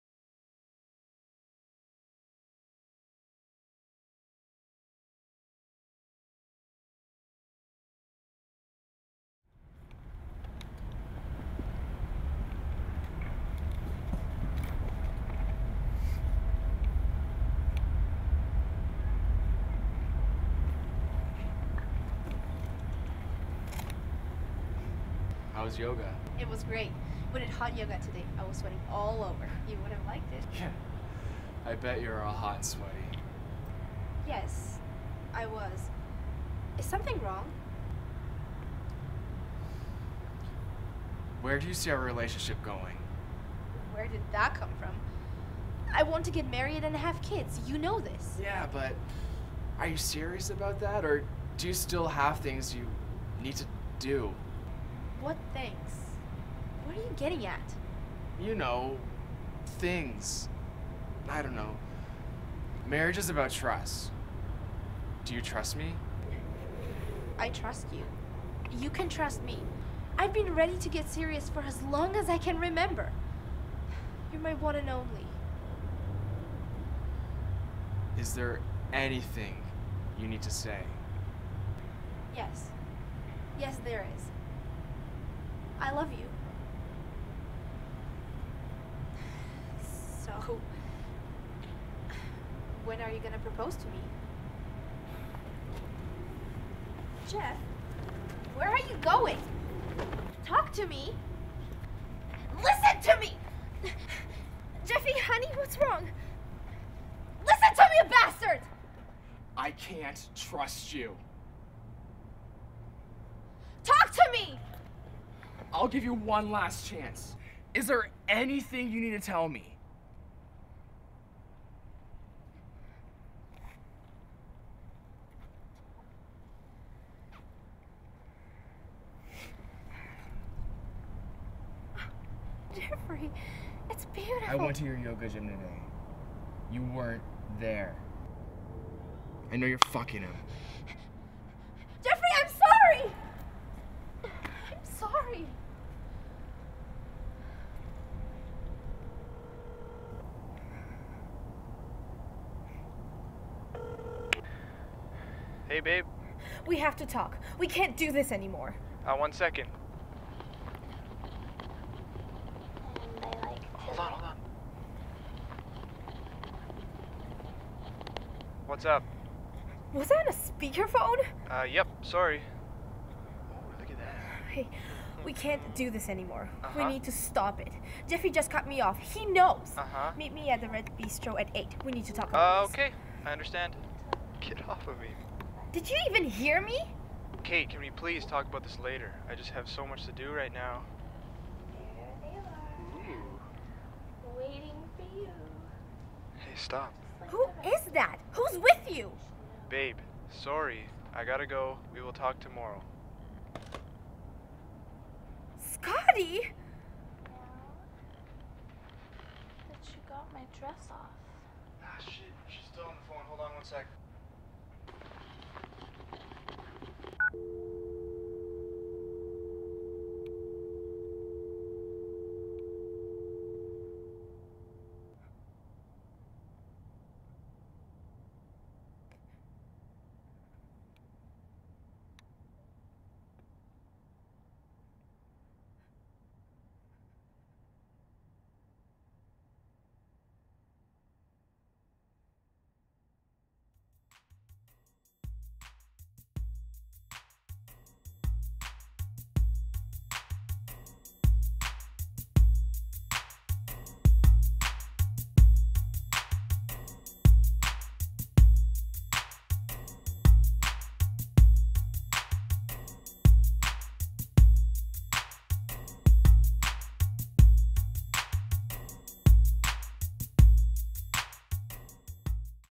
Engagement
The only thing that Kate wants isto get married and start a life. What she doesn't know is that her current situation is discovered by her long time boyfriend Jeff. A non-traditional proposal takes place as things start to spiral out of control.

Wrote/Cast/Produced/Directed/Edited by Nick Lonski.